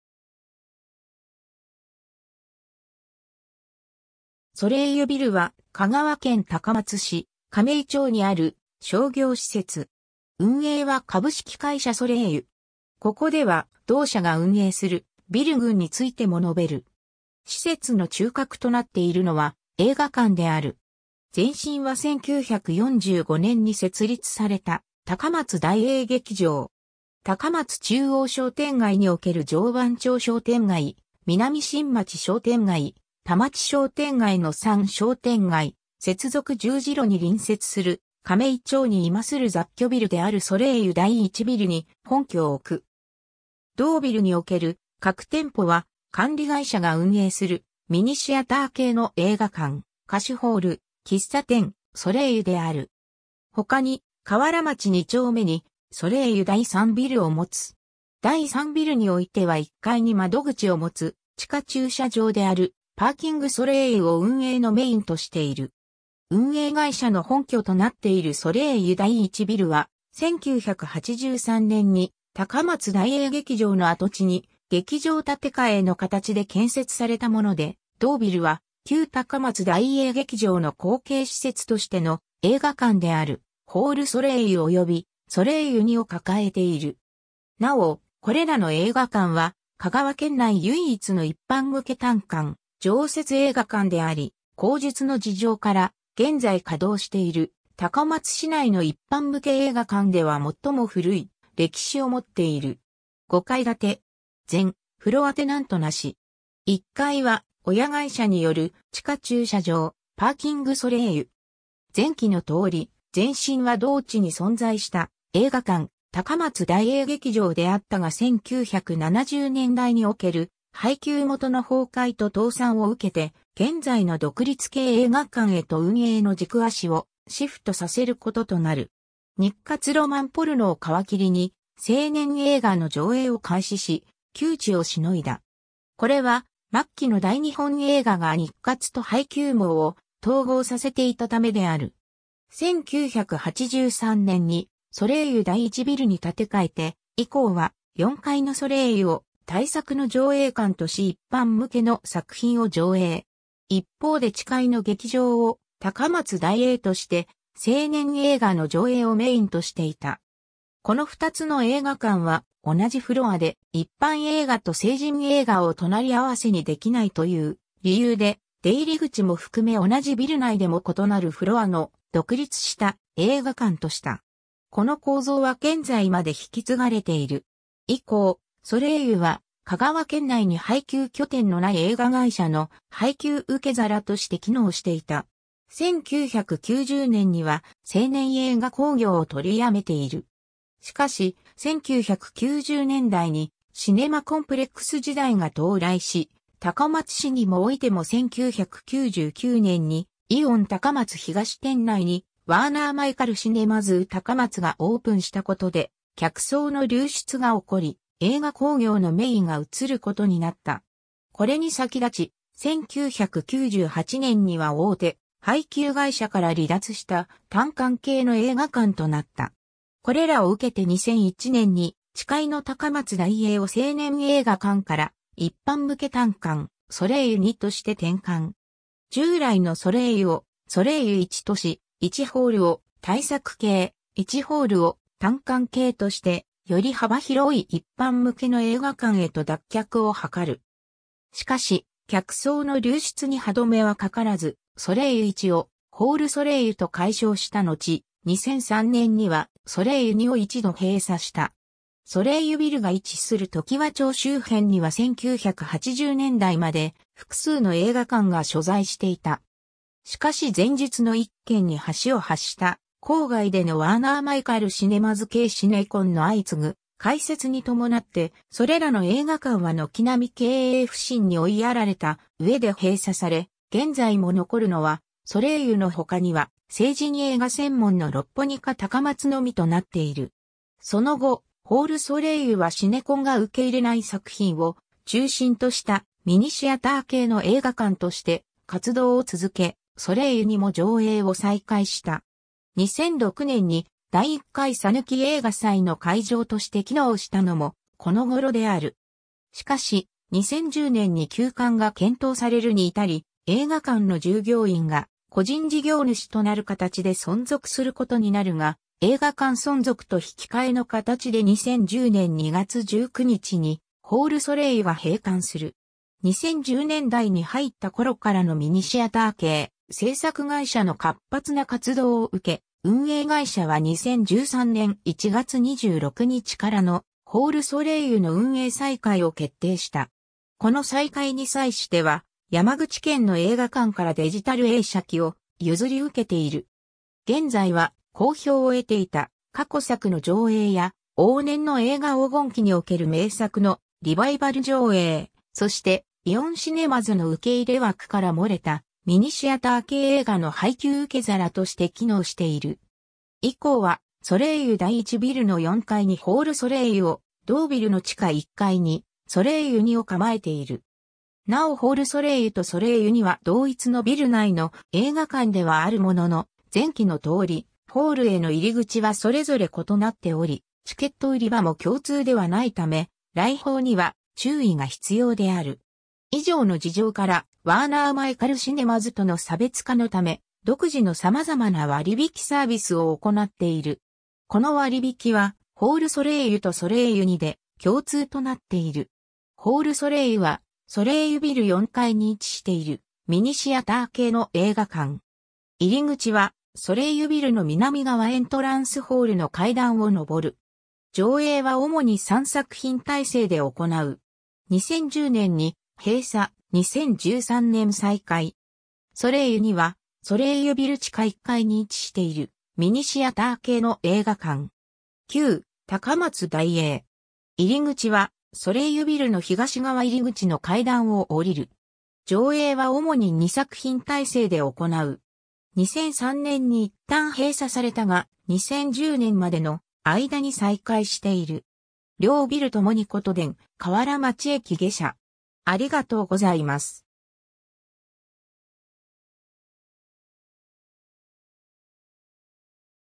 ソレイユビル
ソレイユビルは、香川県高松市亀井町にある商業施設（雑居ビル）。
運営は株式会社ソレイユ。
ここでは同社が運営するビル郡についても述べる。
施設の中核となっているのは映画館である。
 前身は1945年（昭和20年）に設立された高松大映劇場。
 高松中央商店街における常磐町商店街、南新町商店街、田町商店街の3商店街接続十字路に隣接する、亀井町に在する雑居ビルであるソレイユ第1ビル（「ソレイユビル」と表記されている場合は、通常、この第1ビルを指す）に本拠を置く。
同ビルにおける核店舗は管理会社が運営するミニシアター系の映画館・貸ホール・喫茶店「ソレイユ」である。
他に瓦町2丁目にソレイユ第3ビルを持つ。
第3ビルにおいては1階に窓口を持つ地下駐車場であるパーキングソレイユを運営のメインとしている。
 運営会社の本拠となっているソレイユ第1ビルは、1983年（昭和58年）に高松大映劇場の跡地に劇場建替の形で建設されたもので、同ビルは旧高松大映劇場の後継施設としての映画館であるホールソレイユおよびソレイユ2を抱えている。
なお、これらの映画館は香川県内唯一の一般向け単館常設映画館であり、後述の事情から現在稼働している高松市内の一般向け映画館では最も古い歴史を持っている。
 5階建て。
全フロアテナントなし。
1階は親会社による地下駐車場・パーキングソレイユ。
 前記の通り、前身は同地に存在した映画館「高松大映劇場」であったが1970年代における配給元（大日本映画）の崩壊と倒産を受けて現在の独立系映画館へと運営の軸足をシフトさせることとなる。
日活ロマンポルノを皮切りに成年映画の上映を開始し、窮地をしのいだ。
これは末期の大日本映画が日活と配給網を統合させていたためである。
1983年にソレイユ第1ビルに建て替えて以降は、4階のソレイユを大作（徳間・角川傘下時代の大映映画をはじめとする、香川県に配給本拠を持たない大手配給系）の上映館とし一般向けの作品を上映。
一方で地階の劇場を高松大映として成年映画の上映をメインとしていた。
この2つの映画館は「同じフロアで一般映画と成人映画を隣り合わせにできない」という理由で、出入り口も含め同じビル内でも異なるフロアの独立した映画館とした。
この構造は現在まで引き継がれている。
 以降、ソレイユは香川県内に配給拠点のない映画会社の配給受皿として機能していた。
1990年には成年映画興行を取りやめている。
しかし、1990年代にシネマコンプレックス時代が到来し、高松市にもおいても1999年にイオン高松東店（当時は高松サティ）内にワーナー・マイカル・シネマズ 高松（現：イオンシネマ高松東）がオープンしたことで、客層の流出が起こり、映画興行のメインが移ることになった。
これに先立ち、1998年には大手配給会社から離脱した単館系の映画館となった。
これらを受けて2001年に地階の高松大映を成年映画館から一般向け単館(ミニシアター館)ソレイユ2として転換。
従来のソレイユをソレイユ1とし、1ホール(主にはソレイユ1)を大作系、1ホール(主にはソレイユ2)を単館系として、より幅広い一般(特に映画マニア)向けの映画館へと脱却を図る。
しかし客層の流出に歯止めはかからず、ソレイユ1をホールソレイユと改称したのち、2003年にはソレイユ2を一度閉鎖した。
 ソレイユビルが位置する常盤町周辺には1980年代まで複数の映画館が所在していた。
しかし前述の一件に端を発した、郊外でのワーナー・マイカル・シネマズ系シネコンの相次ぐ開設に伴って、それらの映画館は軒並み経営不振に追いやられた上で閉鎖され、現在も残るのはソレイユの他には成人映画専門のロッポニカ高松（旧日活系）のみとなっている。
 その後、ホールソレイユはシネコンが受け入れない作品を中心としたミニシアター系の映画館として活動を続け、ソレイユ2も上映を再開した。
2006年に第1回さぬき映画祭の会場として機能したのも、この頃である。
しかし、2010年に休館が検討されるに至り、映画館の従業員が「個人事業主」となる形で存続することになるが、映画館存続と引き換えの形で2010年2月19日にホールソレイユが閉館する。
 2010年代に入った頃からのミニシアター系制作会社の活発な活動を受け、運営会社は2013年1月26日からのホールソレイユの運営再開を決定した。
この再開に際しては、山口県の映画館からデジタル映写機を譲り受けてい...